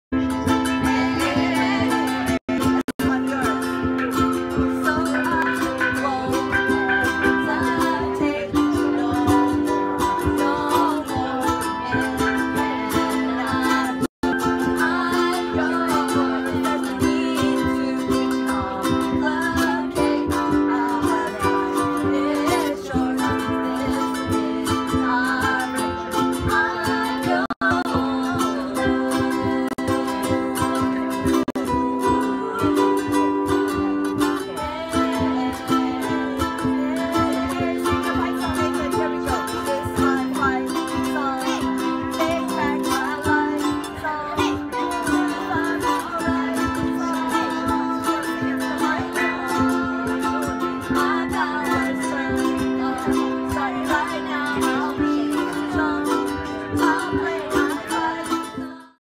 AMB in the Opal Booth La International Ukulele Festival 2017
The Academy of Music for the Blind jamming at our booth at the LA International Ukulele Festival 2017! Great people doing great work! Thanks for stopping by!